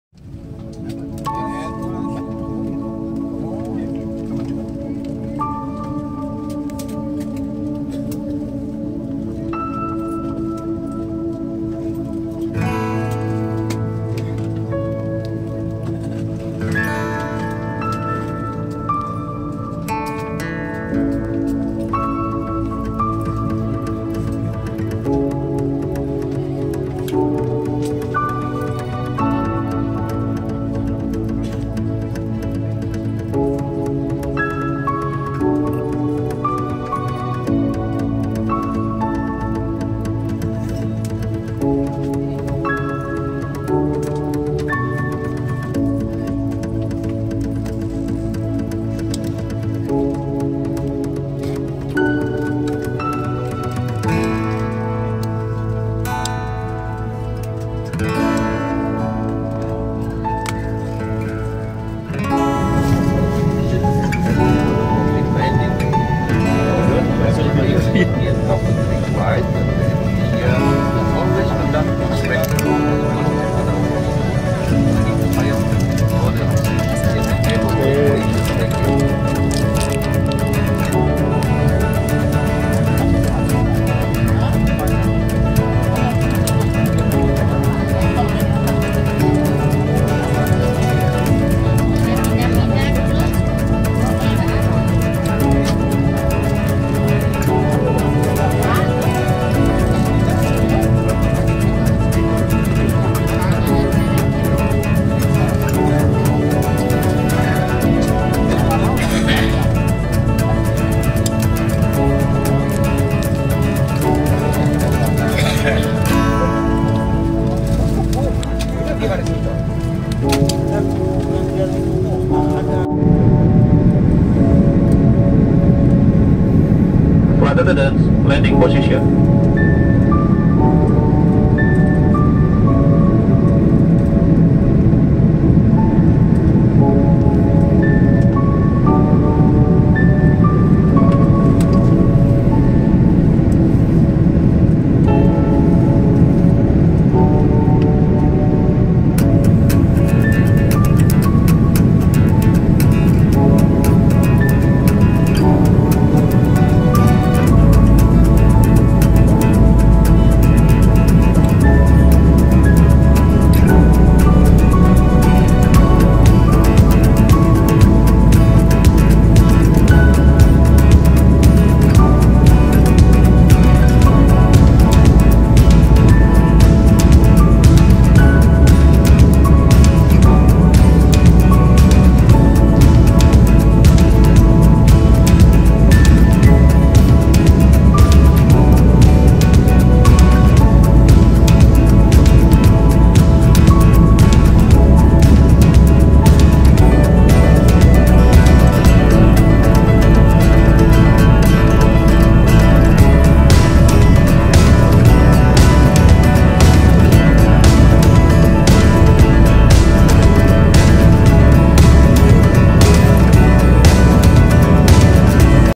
Mencoba Airbus A330-300 | Garuda | Bali - Jakarta | Wajib di Coba
Salah satu pesawat terbaik yang direkomendasikan saat teman-teman bepergian dengan maskapai terbaik Indonesia yakni Garuda Indonesia.

Semoga video ini bermanfaat. Hatur nuhun.